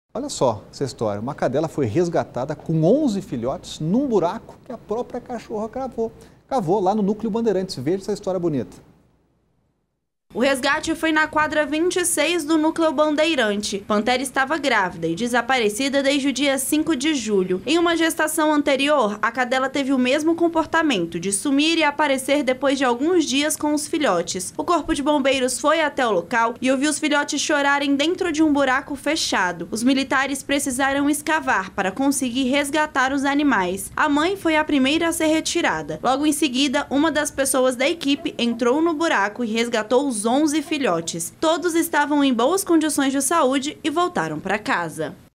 HOMEM QUE DECEPOU MÃO DE PM É PRESO | Band Brasília 07/07/2022
Confira o caso do PM reformado que teve a mão decepada depois de tentar proteger uma mulher, na Asa Norte. A polícia prendeu o agressor, que estava foragido desde o último sábado.